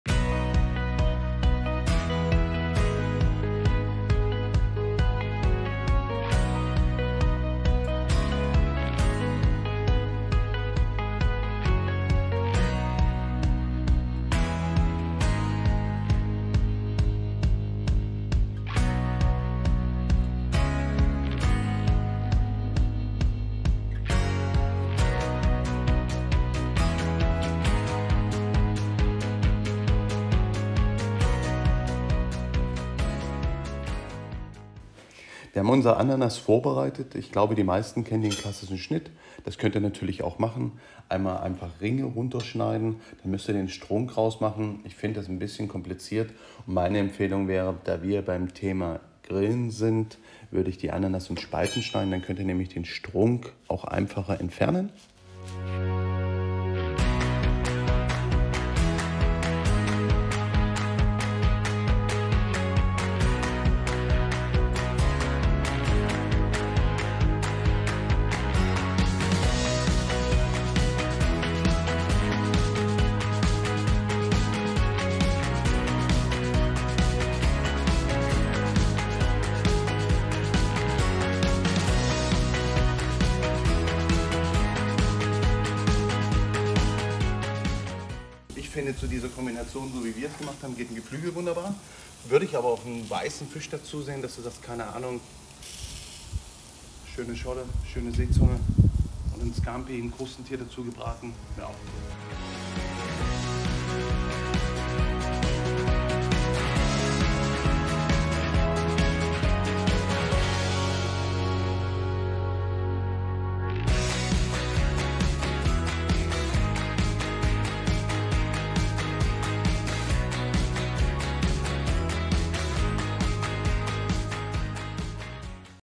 Grillrezept von und mit Zwei-Sternekoch Hendrik Otto
Leckeres Grillrezept mit Ananas, Hähnchenbrust und grünem Spargel.